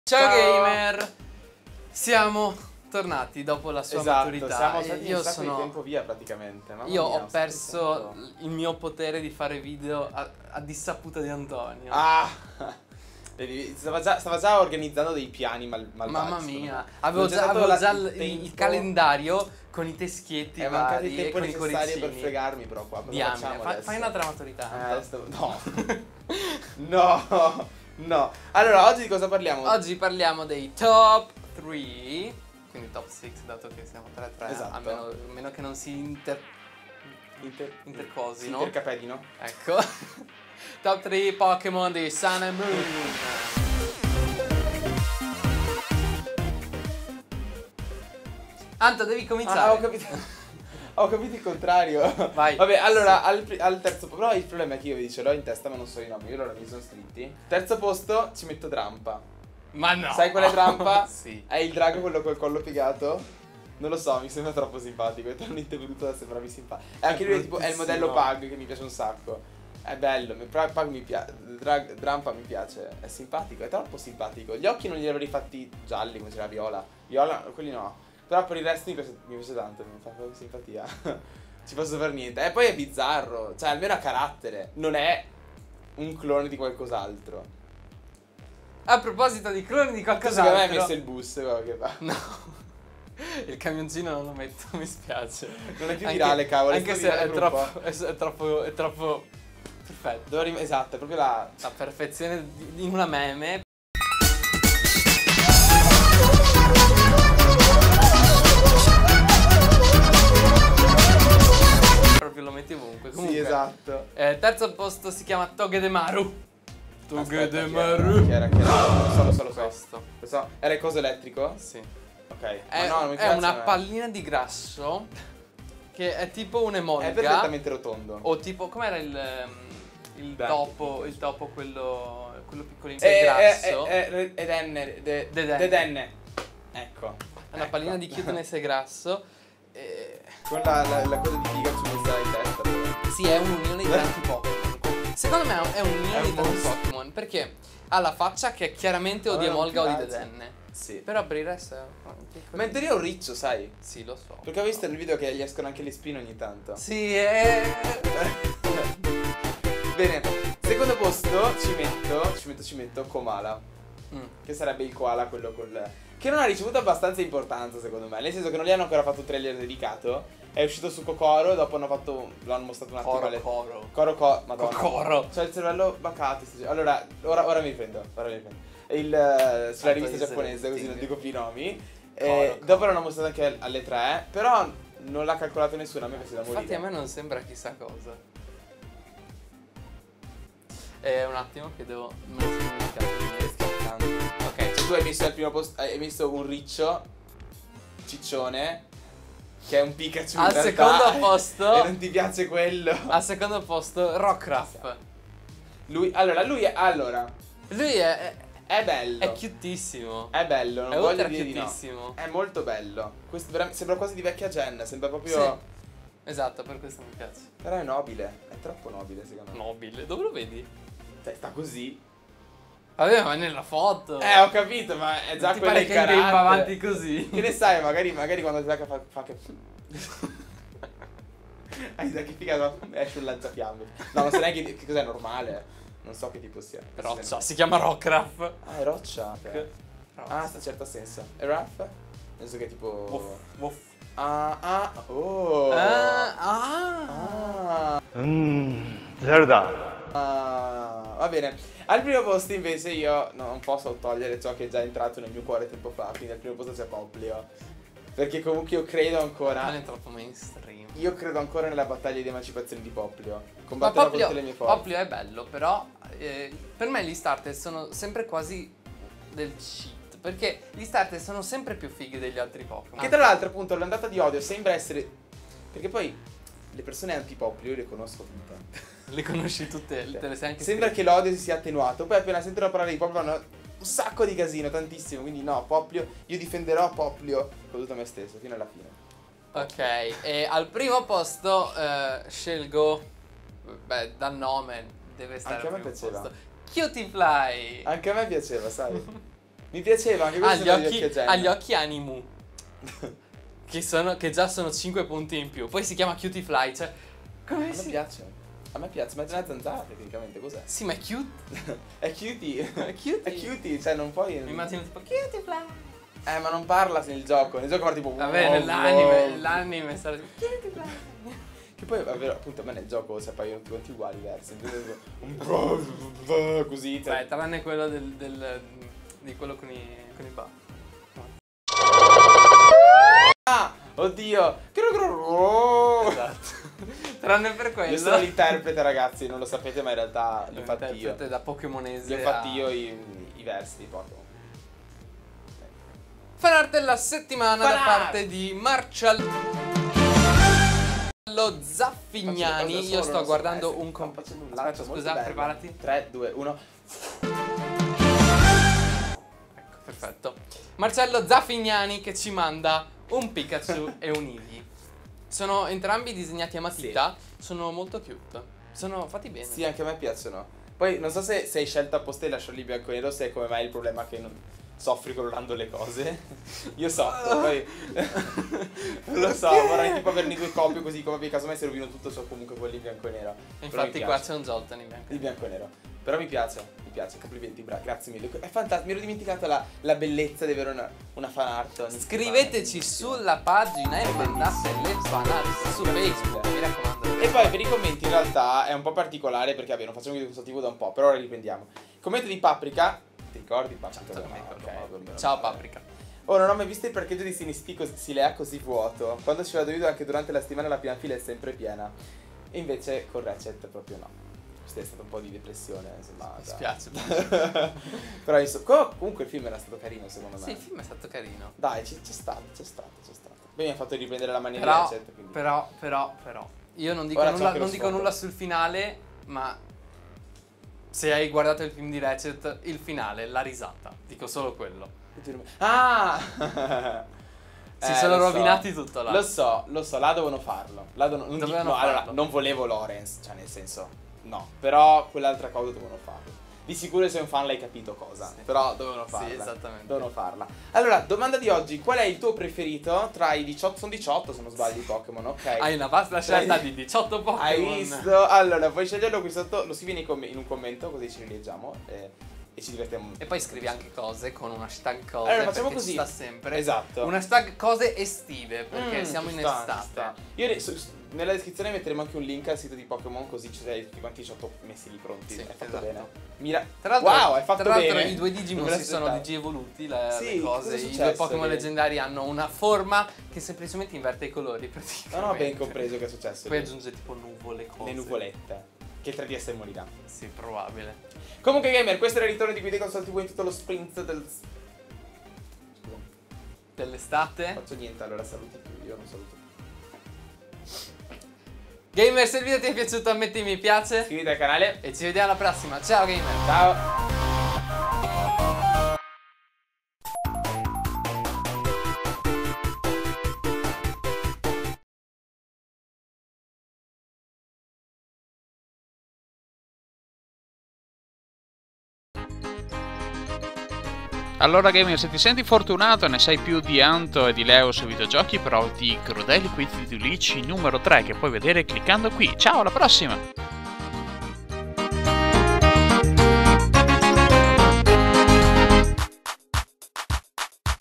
Pokemon sole e luna - Top 3 pokemon cute *_*
Gamer, dovevamo fare un video simile :) GameFreak ha mostrato ormai 18 pokemon di Pokemon Sole e Luna e possiamo quindi già fare una top 3!
Popplio for the win.

-- Link Utili --